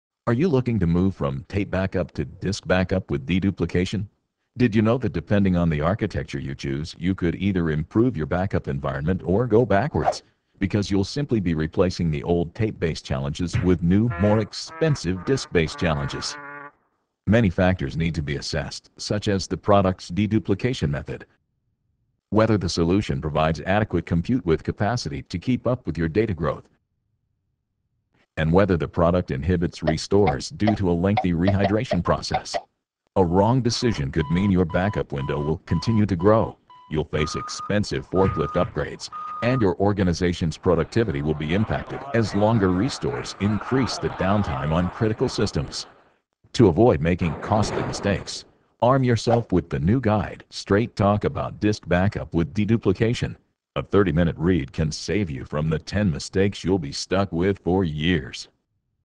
Straight Talk About Disk Backup with Deduplication
Straight Talk About Disk Backup with Deduplication.  Learn what you should know about disk backup with deduplication to avoid 10 costly mistakes!  This eBook is a "must read" if you are you considering a move from tape backup to disk-based backup with deduplication. Available in PDF, Kindle and iPhone formats!